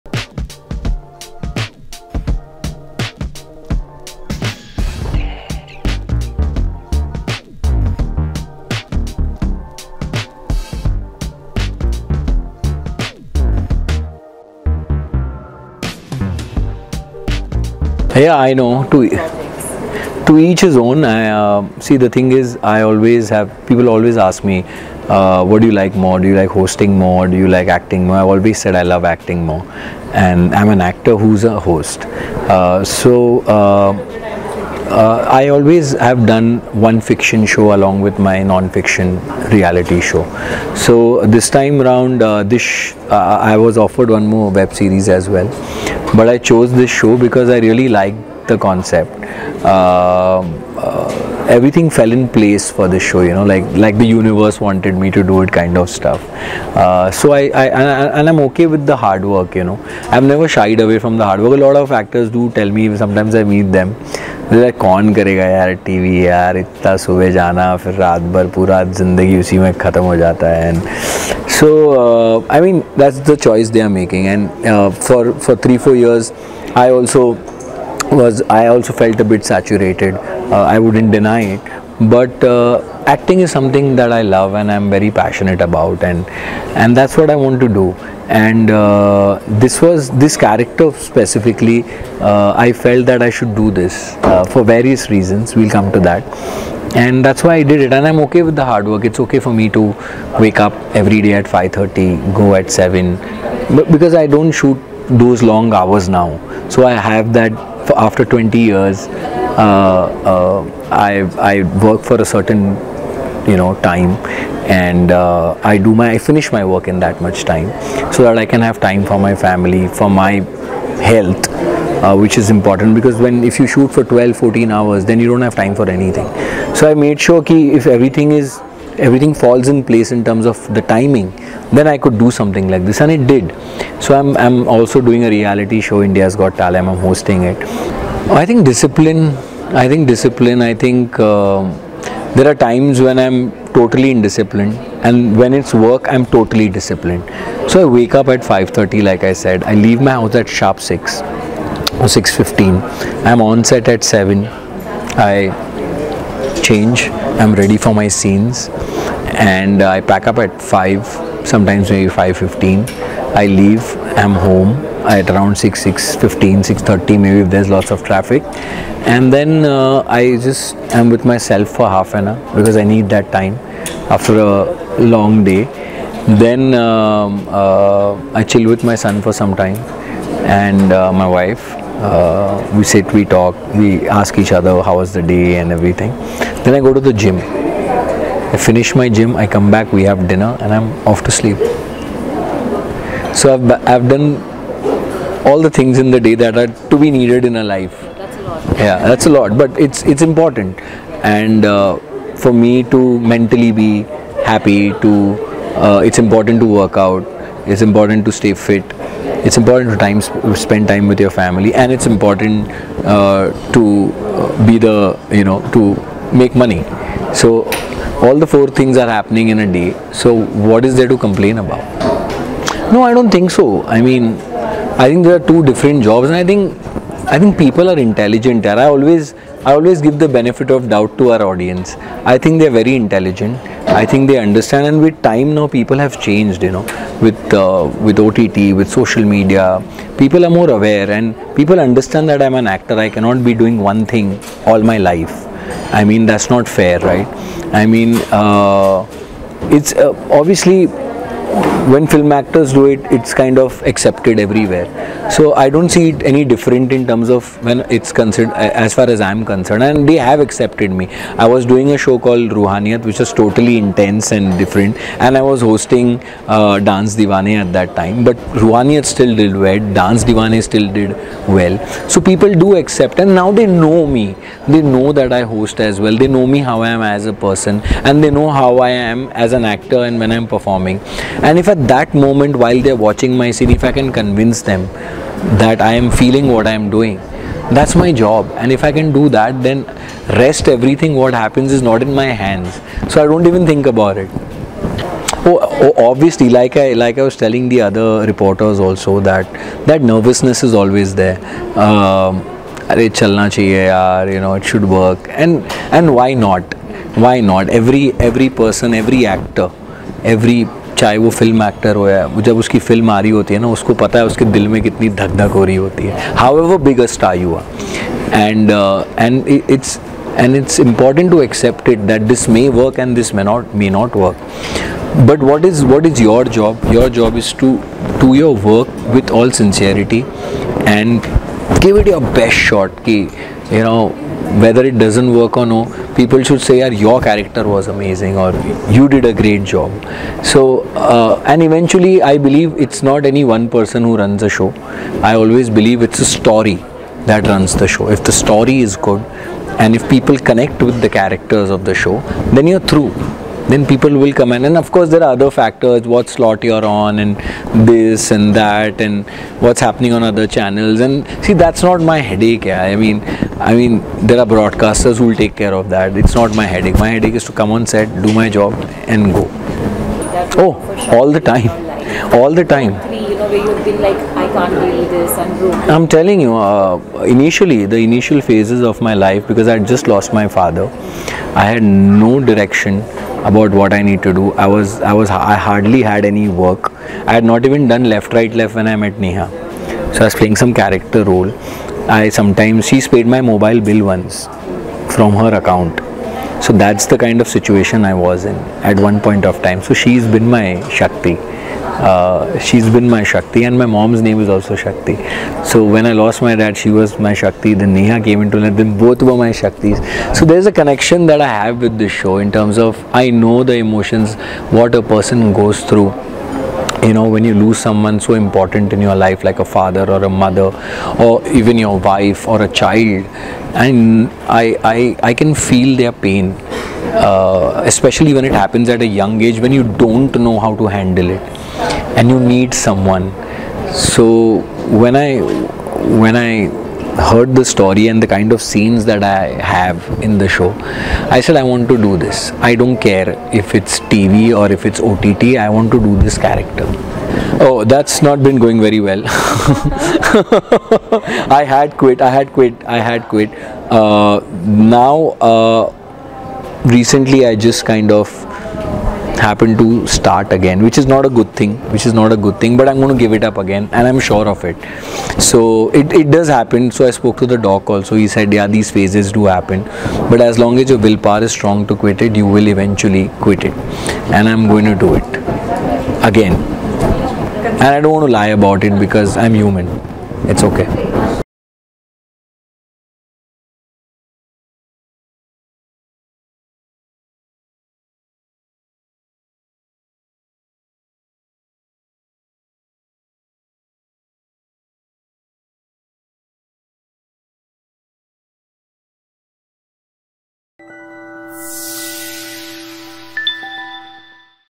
Arjun recalls his struggling days; says, “I couldn’t pay my phone bill once & Neha helped me”
Arjun Bijlani is all set to entertain fans with his fictional journey once again with TV show Pyaar Ka Pehla Adhyaya ShivShakti. The actor will be seen playing the role of Shiv opposite TV fame Nikki Sharma. He got candid and gave an exclusive interview. Arjun opened up about his journey, experience while working, fans’ reaction and more. He also recalled his struggling days and expressed how wife Neha Swami supported him.

Reporter: Gursimran Kaur